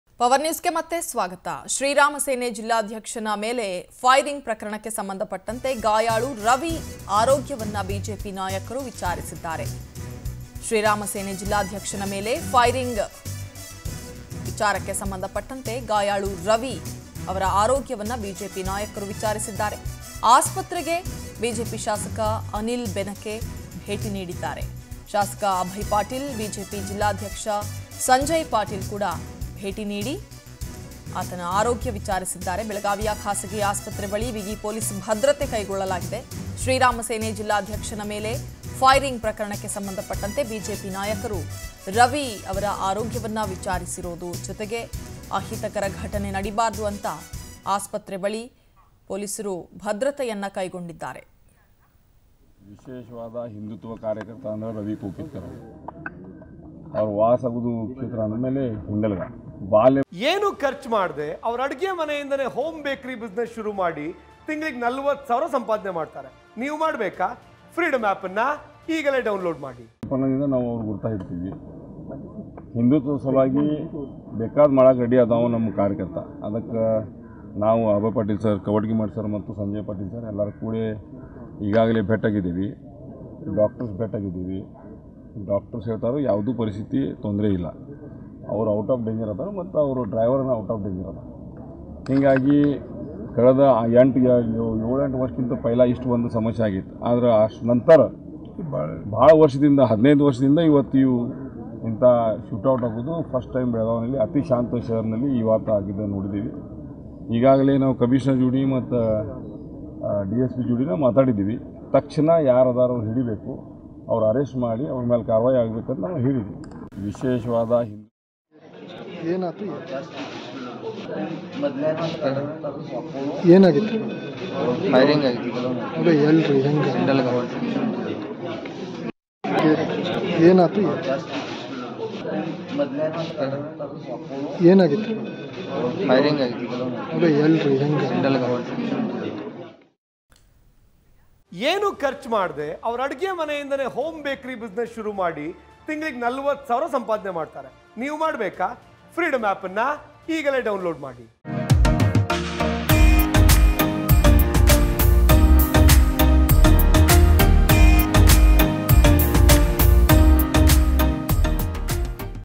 MLA Anil Benake : ಗಾಯಾಳು ರವಿ ಆರೋಗ್ಯ ವಿಚಾರಿಸಿದ BJP MLA | Power TV News
More Updates Keep Following Us